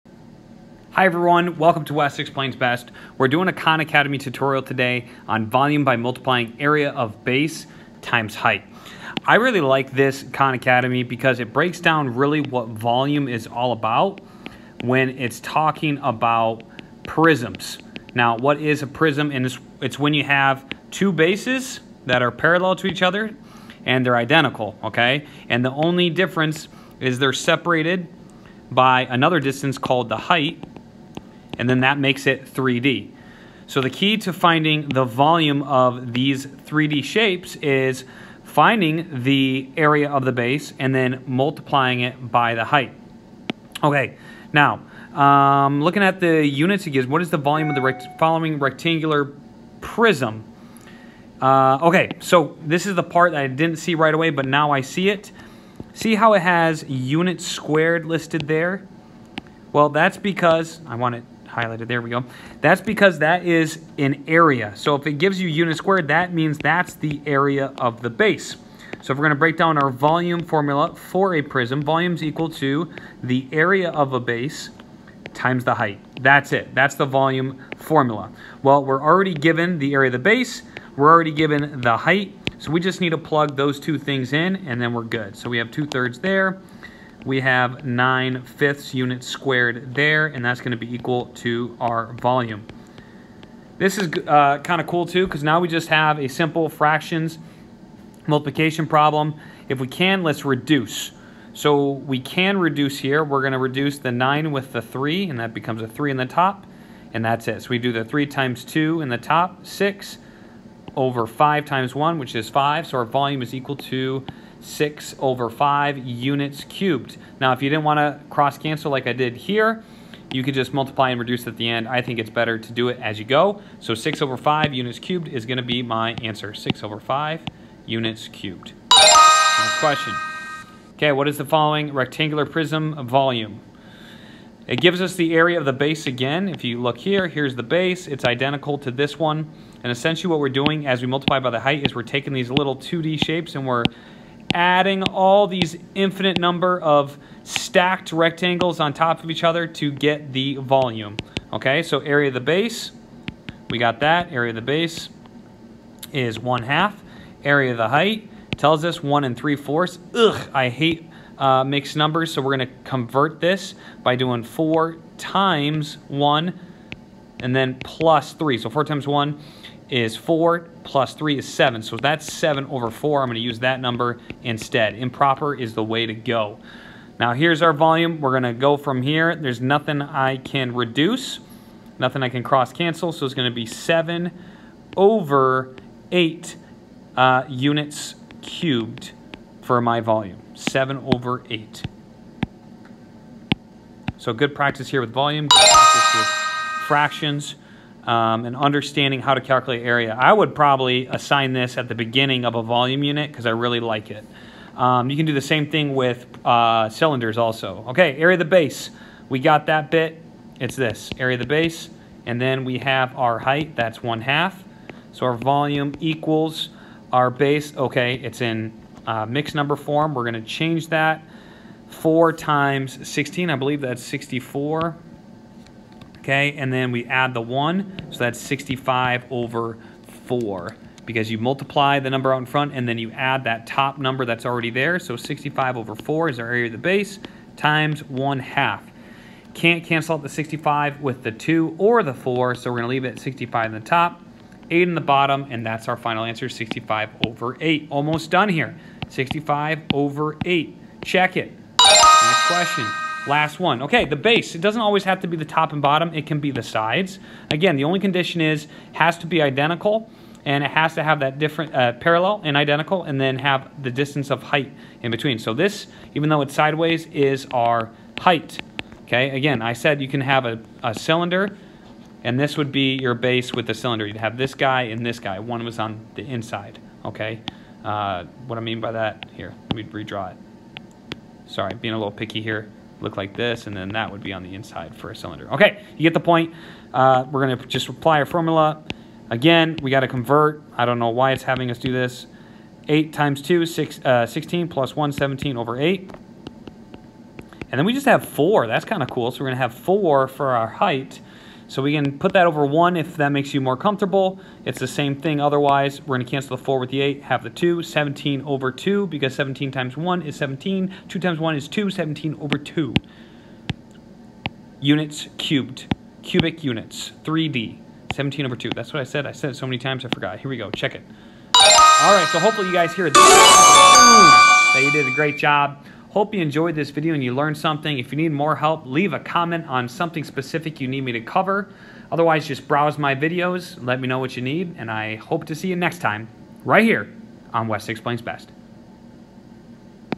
Khan Academy Tutorial: volume by multiplying area of base times height
Title says it all, also includes rectangular prisms, multiplying fractions, and converting mixed numbers to improper fractions

I hope you enjoyed the video! Happiness begins with formation. Formation happens with education. Education starts with you.